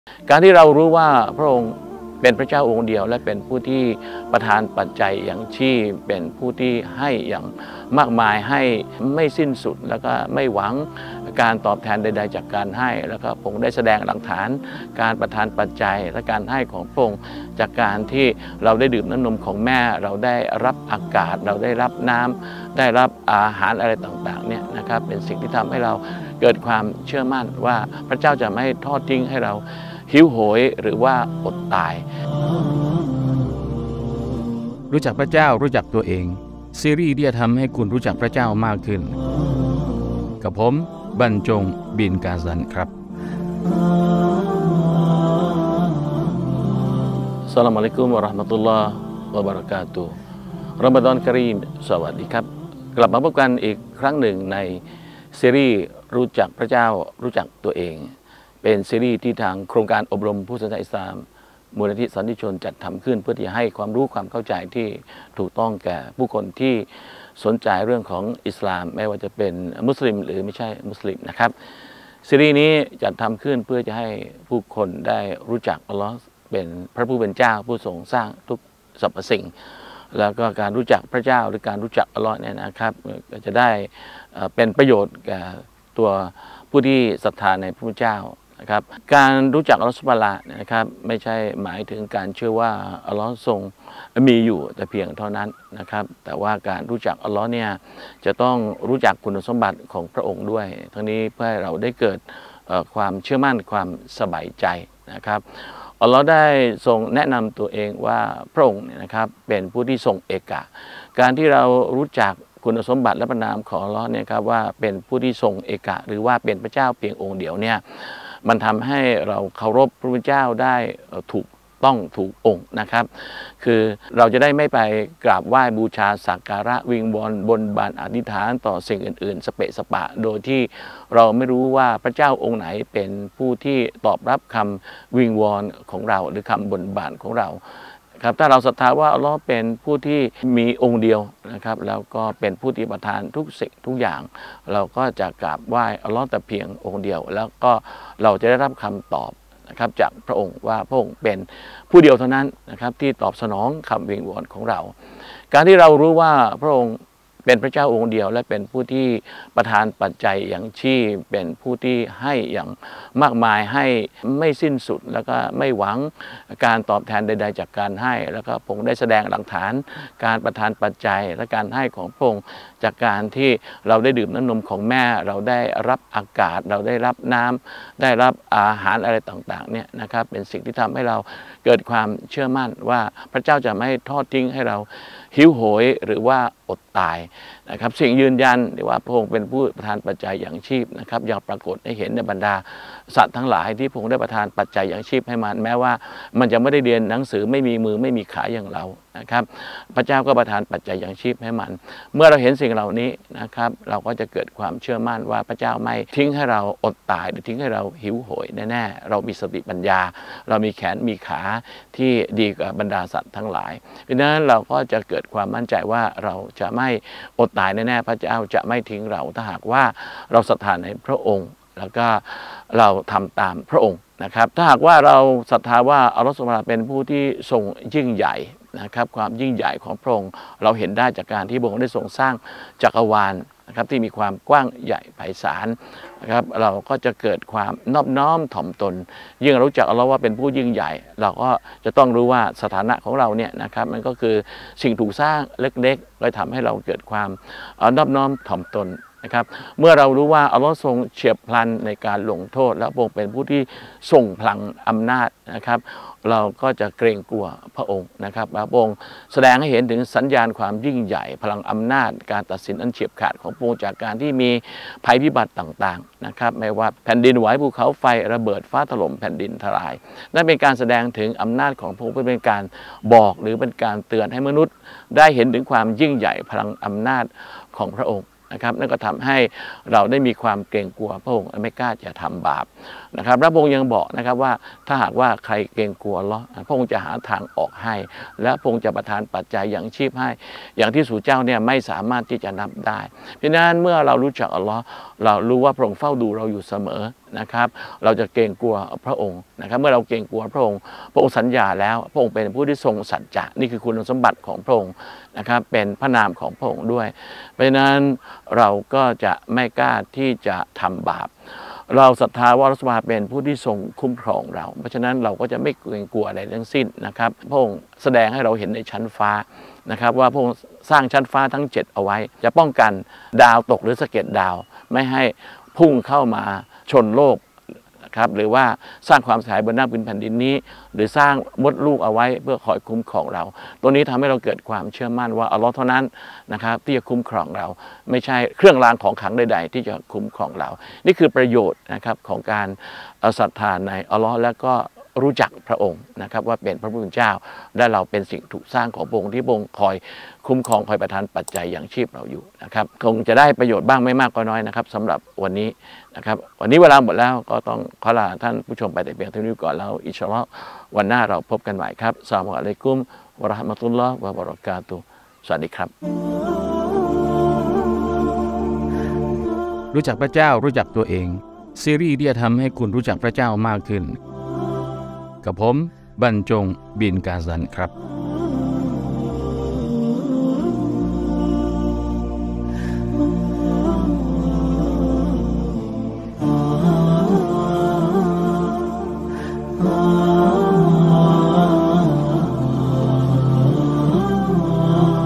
การรู้จักพระนามของพระเจ้า | รู้จักพระเจ้า รู้จักตัวเอง EP.24
อัลลอฮฺ พระผู้เป็นเจ้า ทรงมีพระนามที่สวยงาม 99 พระนาม
และพระองค์ทรงเป็นเช่นนามดังกล่าวทุกประการ
เพื่อให้มนุษย์ได้รู้จักพระองค์มากขึ้น